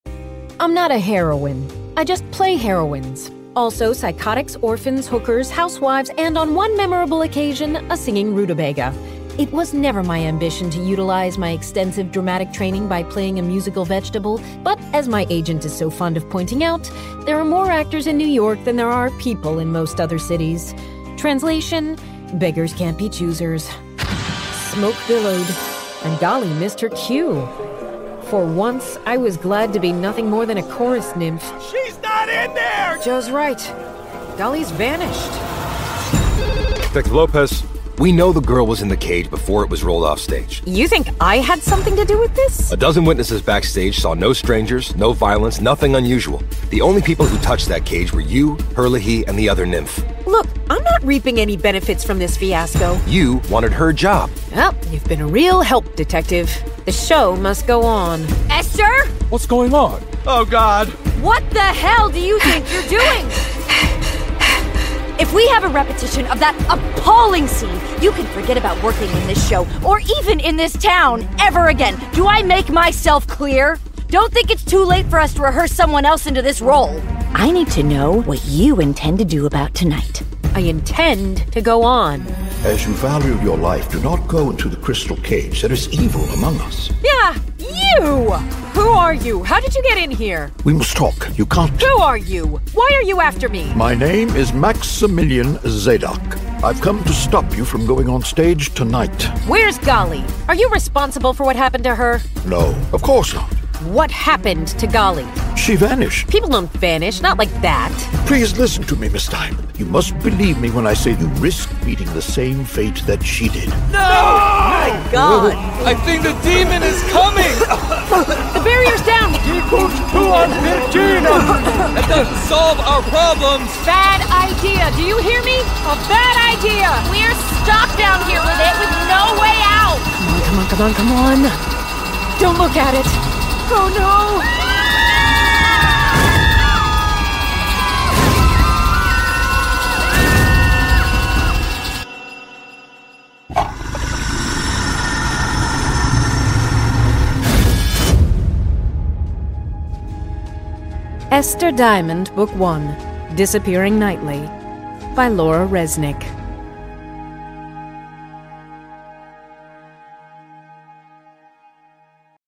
Esther Diamond 1: Disappearing Nightly by Laura Resnick (GraphicAudio Sample)
Esther Diamond, a struggling actress in New York, seems destined to attract supernatural mayhem.

When bizarre magical disappearances disrupt shows around the city, Esther receives a mysterious warning not to go on with her off-Broadway show. Desperate to stay on stage rather than resort to waiting tables, Esther turns to her new BFF, Dr. Maximillian Zadok, a 350-year-old mage whose day job is protecting New York from Evil. Determined to get to the bottom of this mystery without losing her job, Esther and Max team up with a conjuring cowboy, a banker with stage aspirations, and a flock of fearless drag queens. Also on the case is Detective Connor Lopez, a sexy cop who has a thing for Esther, but who fears that she and Max may be a bigger problem than the vanishing performers.

Since the show must go on—and the astronomical rent must be paid—Esther, Max, and their friends pursue Evil to its lair in their fearless determination to find the missing performers and restore harmony to the city that never sleeps.